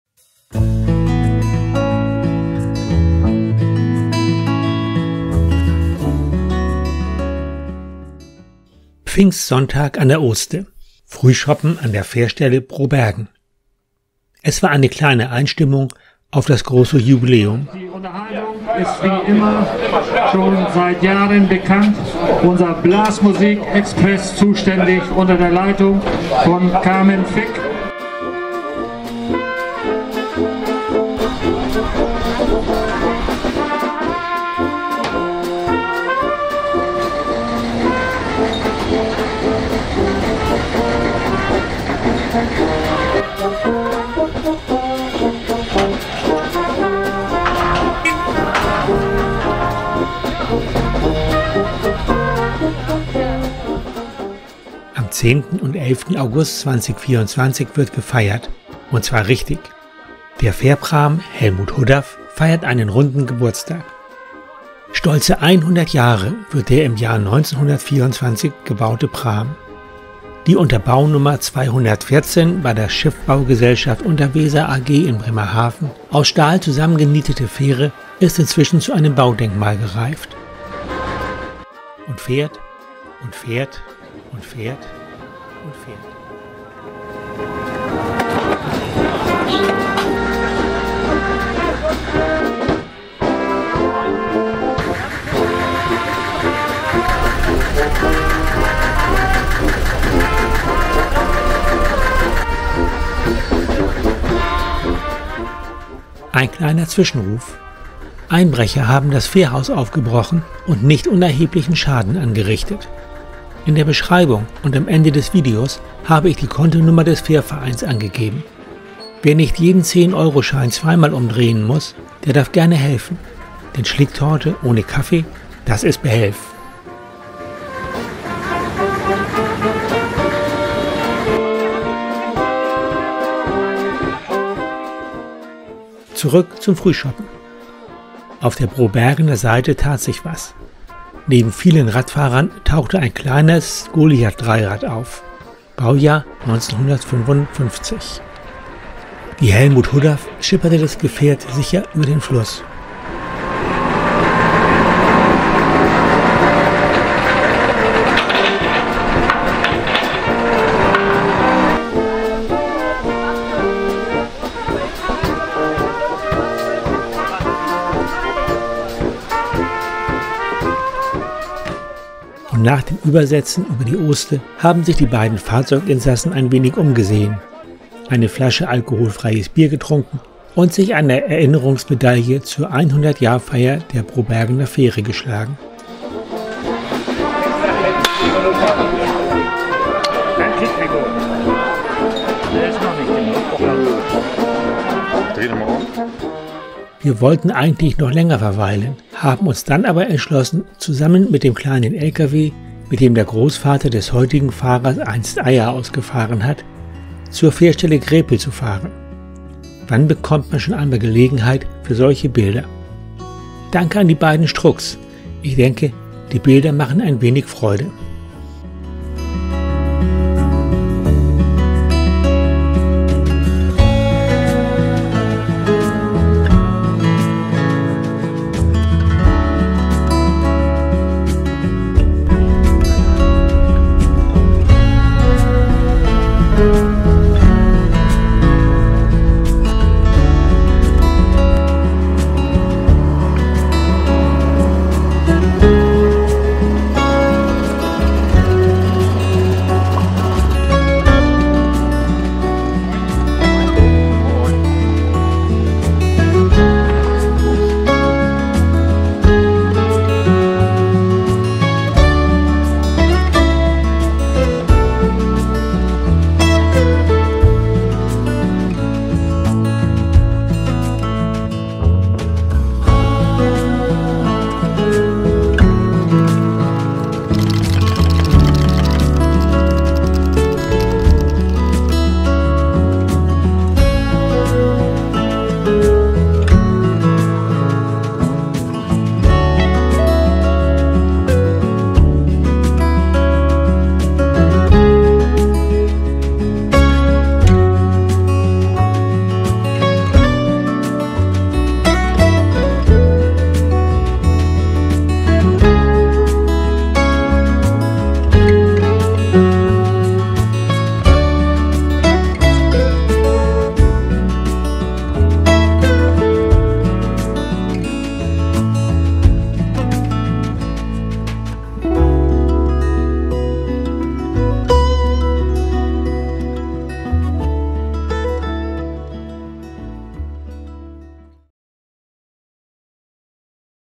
🔴 Pfingsten an der Oste
Der Fähr- und Geschichtsverein Brobergen und Umgebung e. V. ist Opfer von Einbrechern geworden.
Wer nicht jeden 10,– €-Schein zweimal umdrehen muss, darf gerne etwas spenden, damit wir auch weiterhin eine Tasse Kaffee zur Schlicktorte bekommen können ;-).

Hier ist die Bankverbindung:

Fähr- und Geschichtsverein Brobergen und Umgebung e. V
IBAN: DE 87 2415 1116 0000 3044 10
BIC: NOLADE21STK